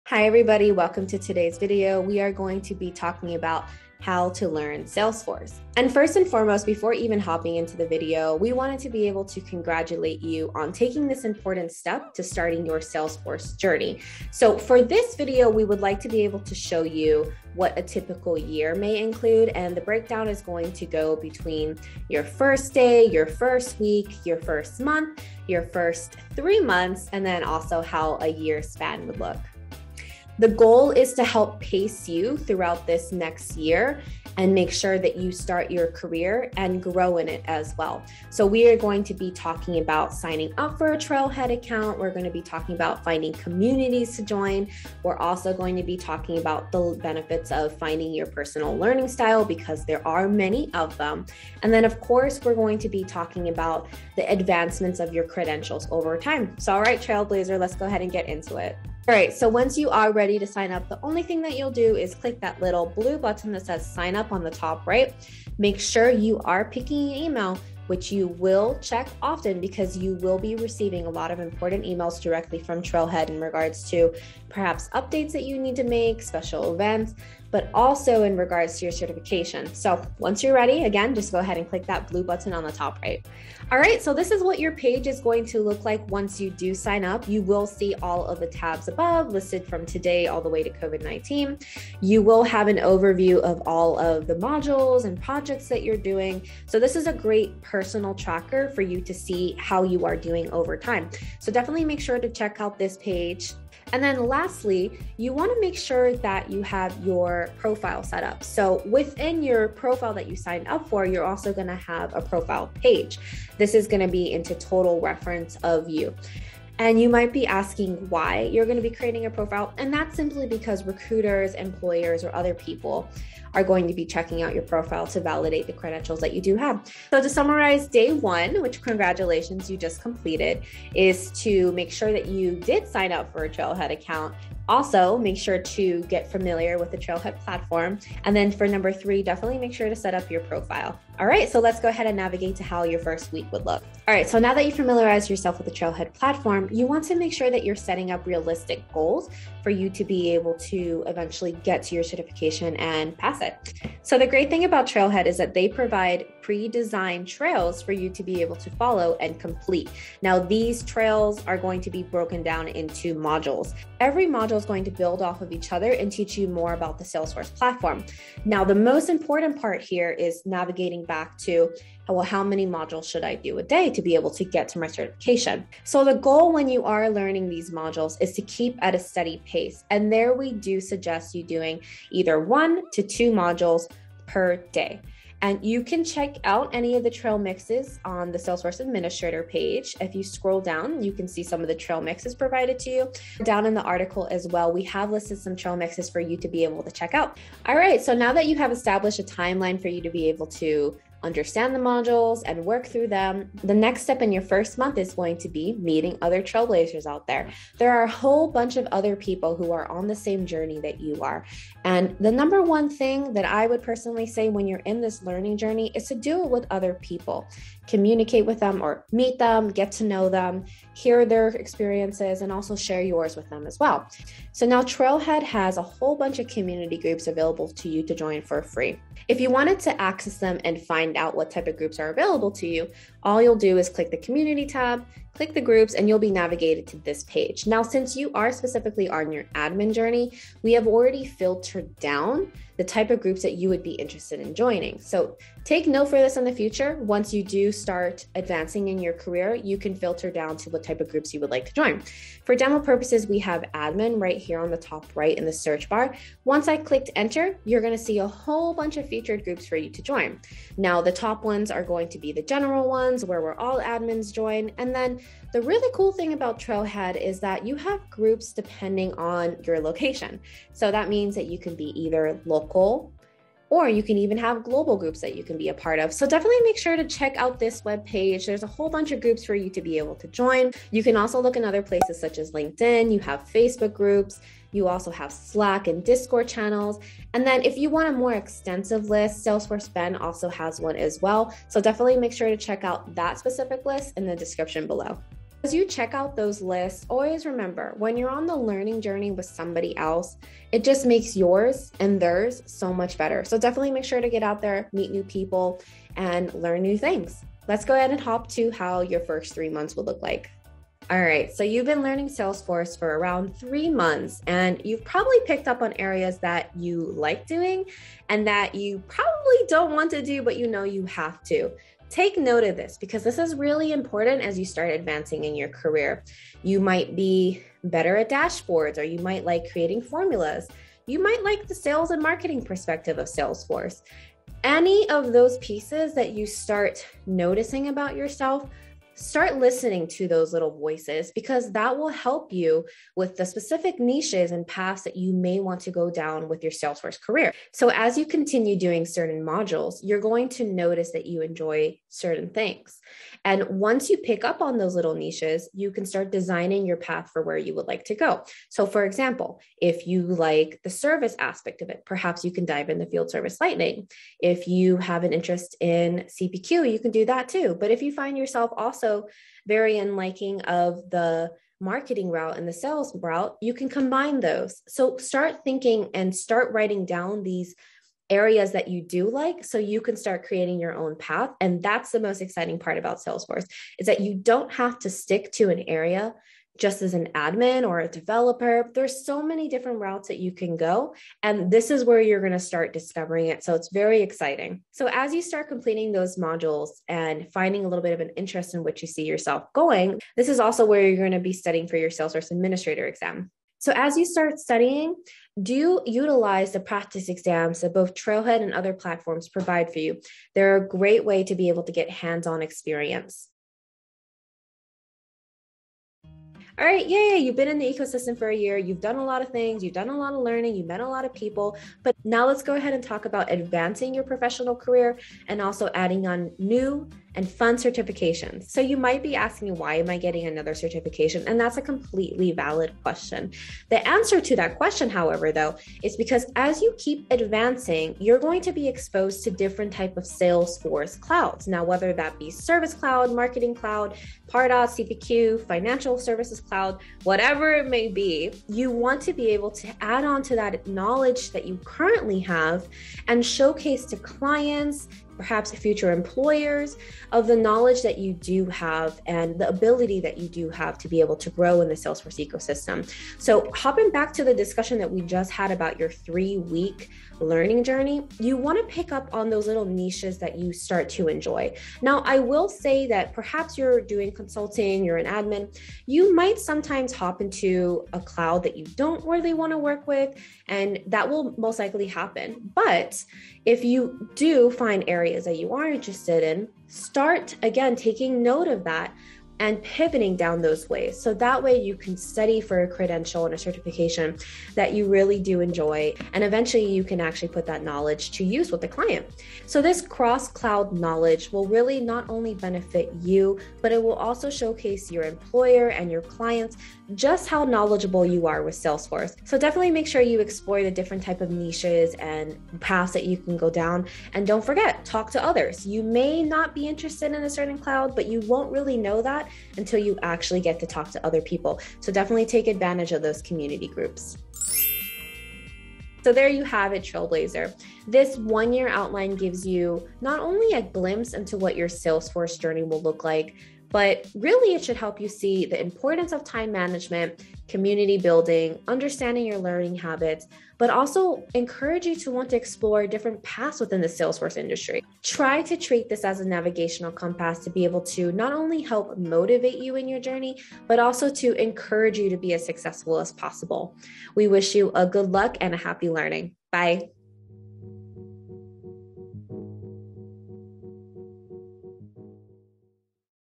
How to Learn Salesforce [+ Video]
You’ve decided to pursue a career working with Salesforce – congratulations! As you start gearing up for this journey, you might want to know what a typical year will look like and set goals to help pace yourself towards success.

This includes registering for a Trailhead account to discover and hone your learning style, finding communities to join, and learning the benefits of earning new credentials throughout your Salesforce journey. All right let’s get you started on your way to becoming a Trailblazer!